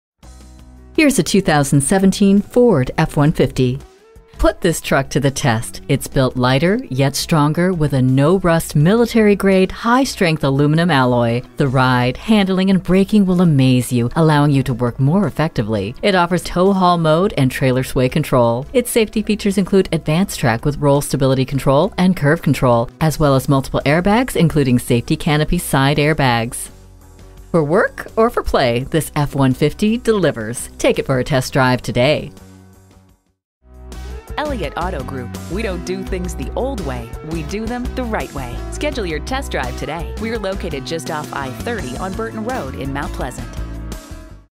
New 2017 Ford F-150 Mt Pleasant TX Sulphur Springs, TX F5353 - SOLD
Year: 2017
Make: Ford
Model: F-150
Trim: 4WD SUPERCREW
Engine: 2.7L V6
Transmission: Automatic 6-Speed
Color: Race Red
Interior: Black w/Unique Sport Cloth 40/Console/40 Front-Sea
Stock #: F5353
VIN: 1FTEW1EP5HKD16087

Address:
2045 Burton Rd.